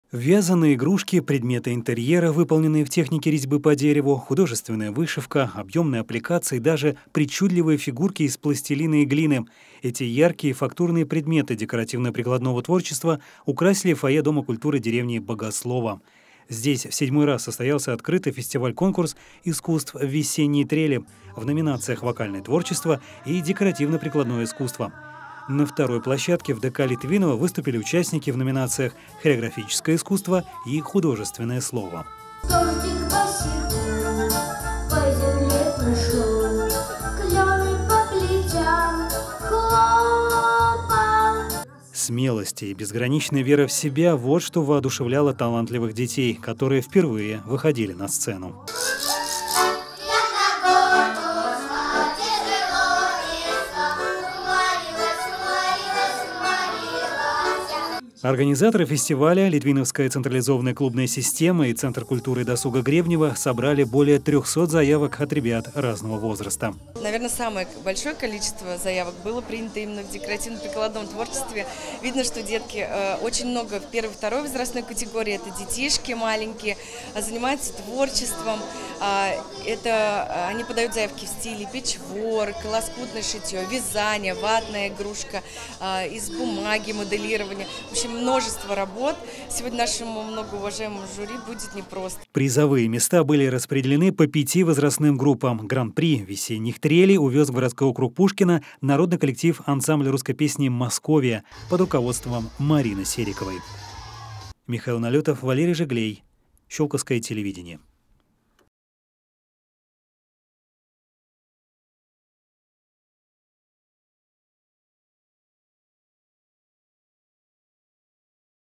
В Богослово состоялся седьмой открытый фестиваль-конкурс искусств «Весенние трели»
Жанров многообразие. В доме культуры деревни Богослово состоялся седьмой открытый фестиваль-конкурс искусств «Весенние трели». Его участники продемонстрировали способности в декоративно-прикладном творчестве и вокальные навыки. Смотрите репортаж.